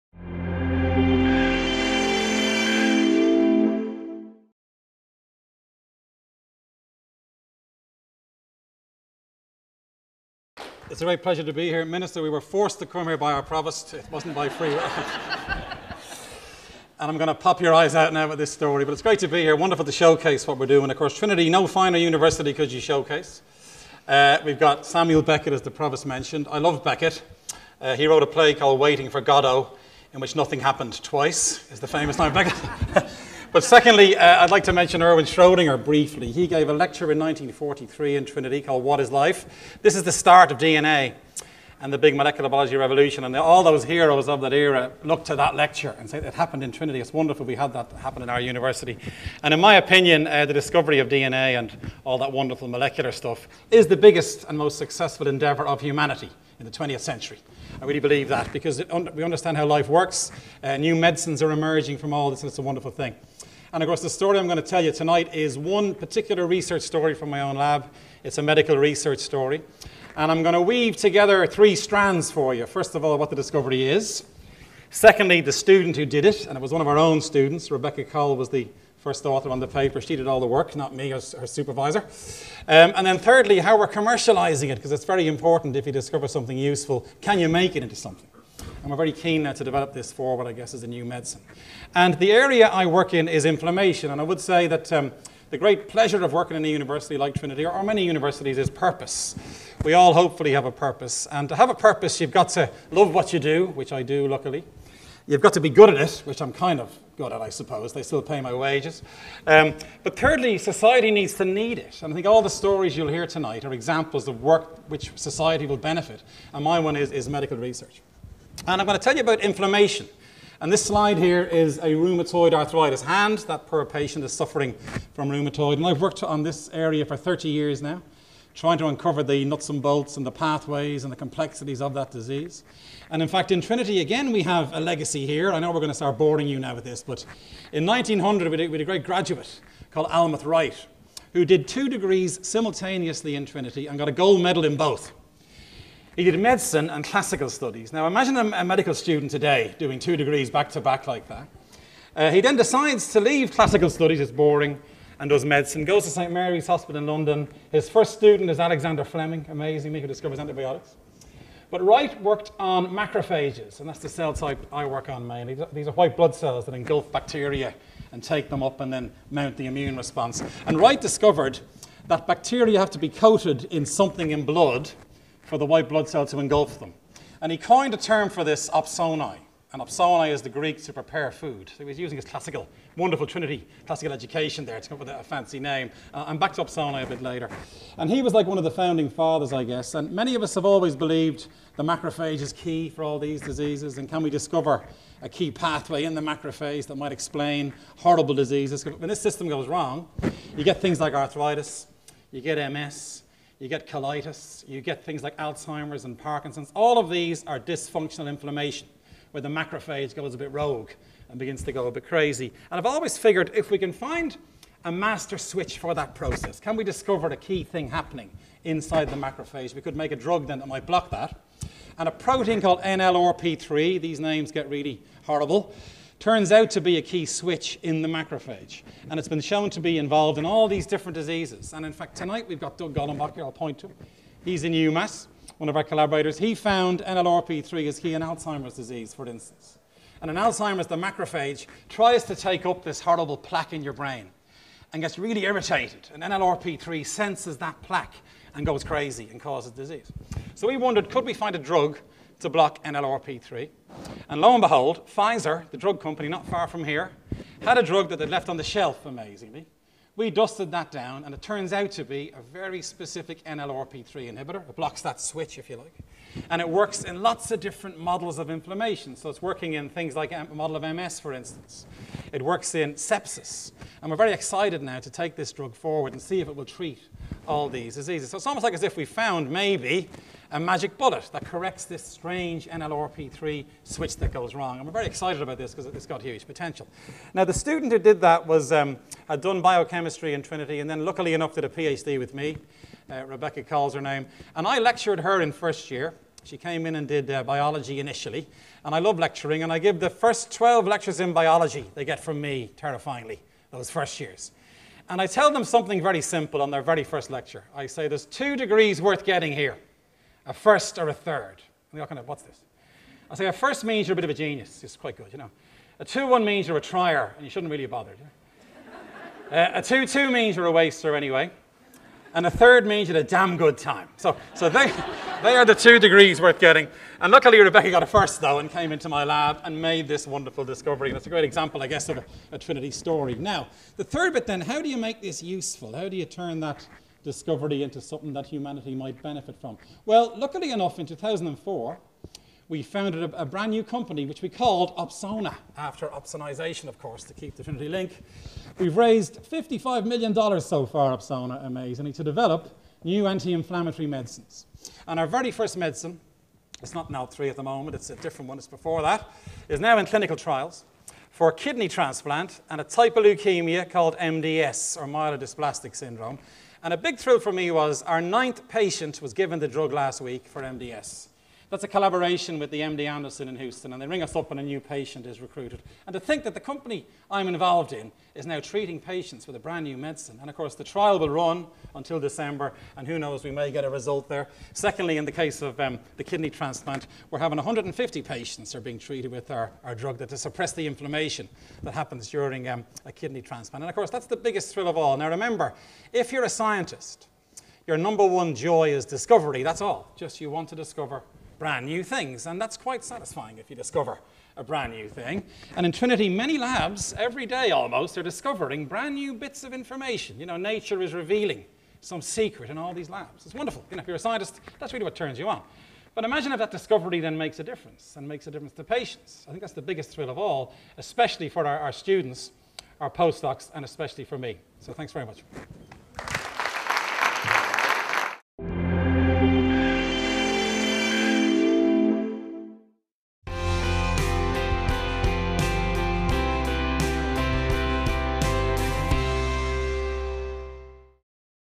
Prof. Luke O’Neill speaks at Trinity research showcase in Boston, MA
Professor Luke O’Neill leads the Inflammation Research Group in the School of Biochemistry and Immunology, in the Trinity Biomedical Sciences Institute. He has spent almost 30 years investigating the intricacies of the inflammatory response and the overall mission of the O’Neill lab is to make world-beating discoveries in inflammation that might prove useful to patients. Some of Trinity’s top academics presented their research in an engaging TEDx style. The event took place in District Hall in Boston on May 28th 2015 and was opened by Ireland’s Minister for Education, Jan O’Sullivan.